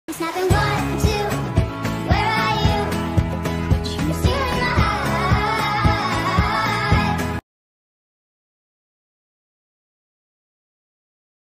Lego monkie kid 💙Jealous mk au💙 ⚠️Read the descrtion to understand more
Mk is jealous because he doesn't have any parents and always got left behind .